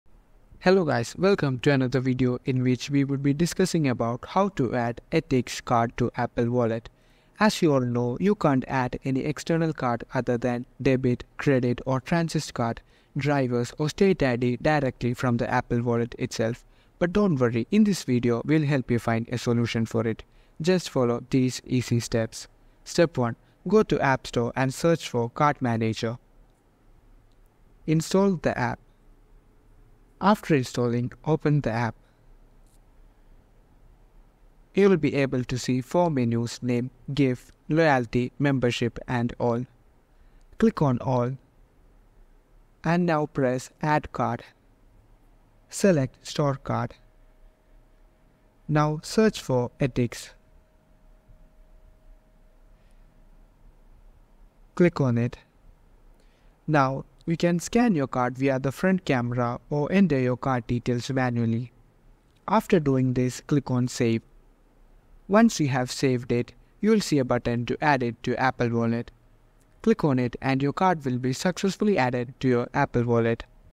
How To Add Etix To Apple Wallet Easy
In this informative YouTube video, we will guide you through the simple steps to add Etix to your Apple Wallet effortlessly. Whether you're a frequent traveler or someone who loves attending events, having your tickets conveniently stored in your Apple Wallet can make your life much easier. We will demonstrate the process step by step, ensuring that you can easily follow along and add your Etix tickets to your Apple Wallet hassle-free.